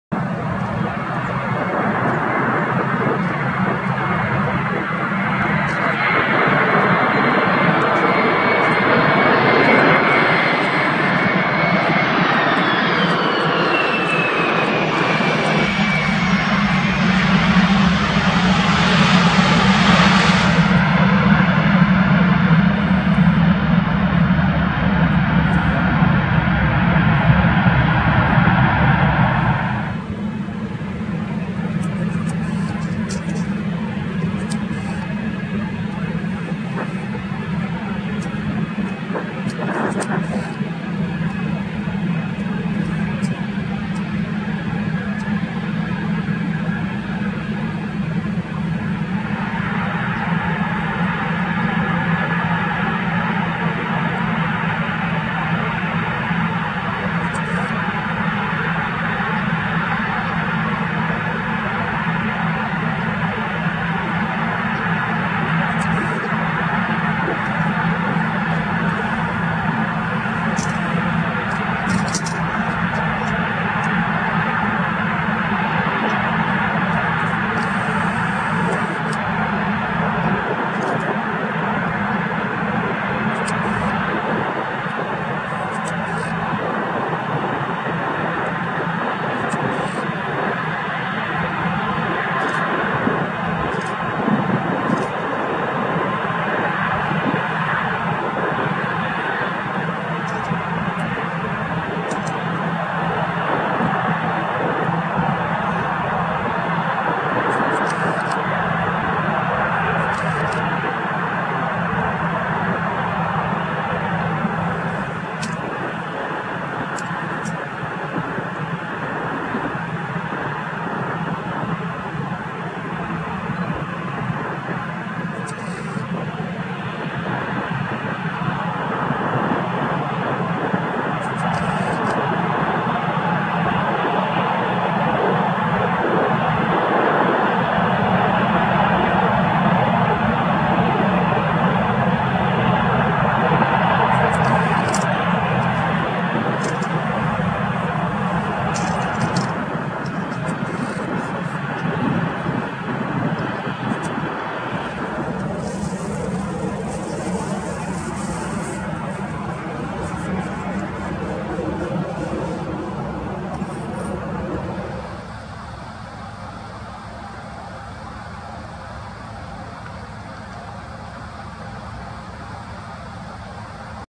Transaviaexport Cargo Boeing 747-329F Landing At Belgrade Airport
This morning we had a very rare and special plane here in Belgrade. This aircraft arrived from Minsk shortly after 7am and departed to Prince Sultan Air base at around 10:20am. Enjoy!

Minsk(MSQ)➡Belgrade(BEG)

Information about the aircraft:

Registered Operator: Transaviaexport Cargo Airline
Aircraft Type: Boeing 747-329(SF)
Registration: EW-465TQ
Engines: 4x GE CF6-50E2
Construction Number(MSN): 24837
Delivered to Transaviaexport in 2016.

Date and time of recording: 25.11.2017/7:08am